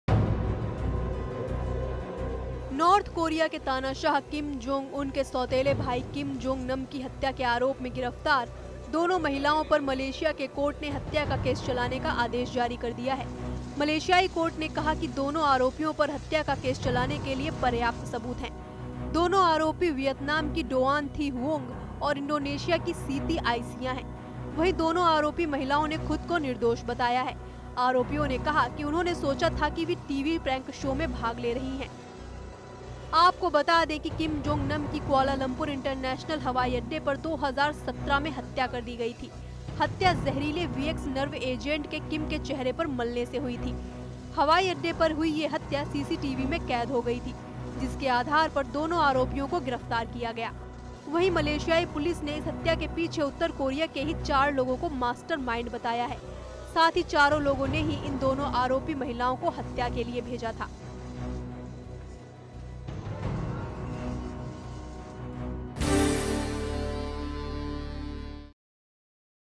किम के भाई की हत्या का राज खुलेगा | Duniya Tak
किम जोंग के भाई की हत्या के मामले की आरोपी महिलाओं पर फैसला सुनाएगी अदालत..

The court will decide the case of accused women for murdering Kim Jong's brother..